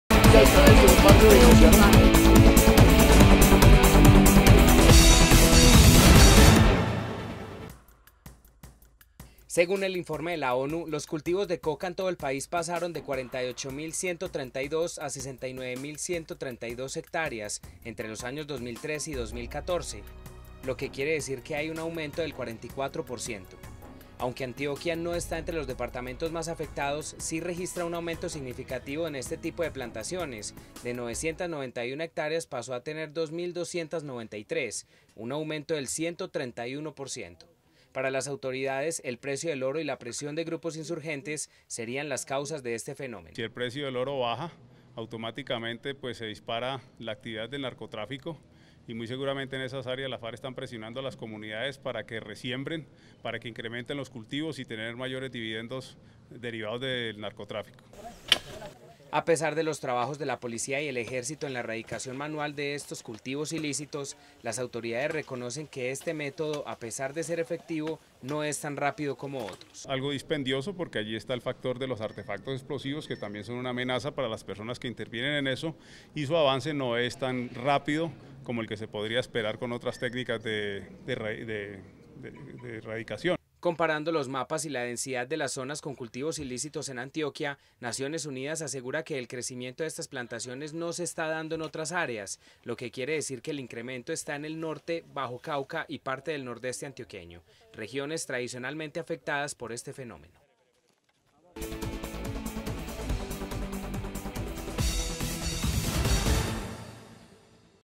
Informe de Naciones Unidas revela incremento del 131% en cultivos de coca en Antioquia
Este jueves la Organización de Naciones Unidas publicó su informe anual sobre el monitoreo de los cultivos de coca en todo el territorio colombiano.

El departamento de Antioquia, que en los últimos años tenía un descenso considerable en las áreas con cultivos ilícitos, registró un aumento del 131% en comparación con el periodo entre 2013 y 2014.

Para las autoridades, el precio del oro y la presión de grupos armados ilegales serían las causas de este incremento en cultivos de coca.